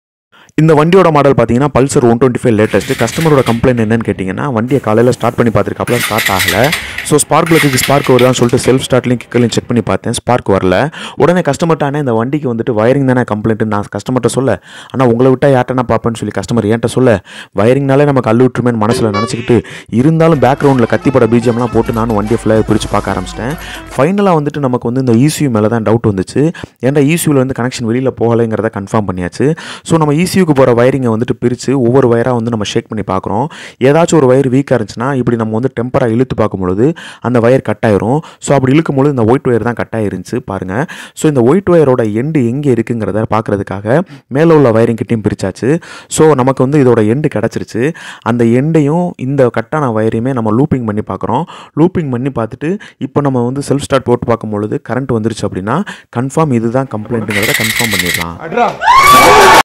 Pulsar125 current complaint🔥| Bike care 360|Tamil
Finding wiring solution about pulsar 125...
P.S MOTORS,
KARUNGALAKUDI,
MELUR TALUK,
MADURAI-DT.-625101.